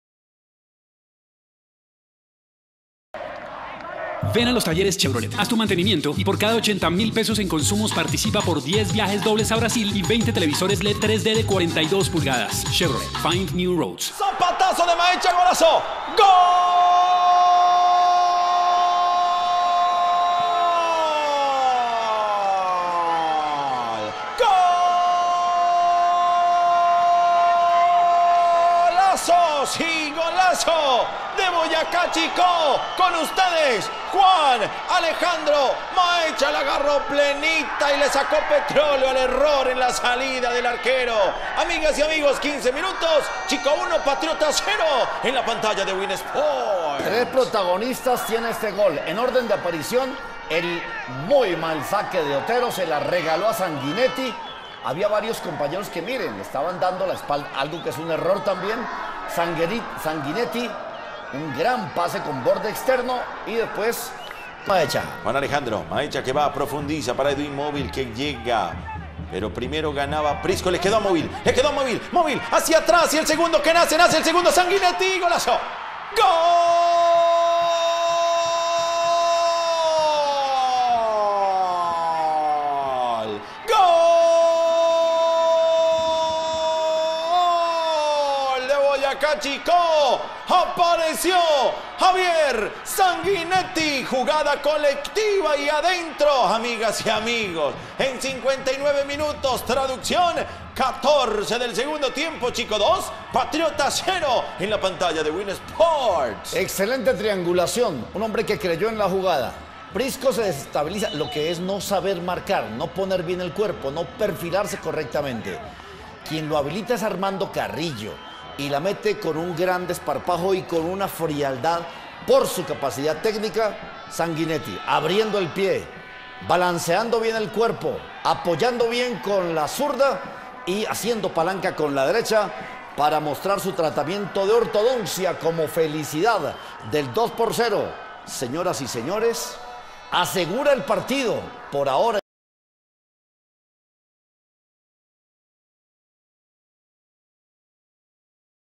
(Resumen) Boyacá Chicó 2 - 0 Patriotas. Fecha 17 Liga Postobón 2014-I - Win Sports
Goles de Juan Mahecha y Javier Sanguinetti (Chicó)
Boyacá Chicó 2 - 0 Patriotas.
Fecha 17 Liga Postobón 2014-I - Win Sports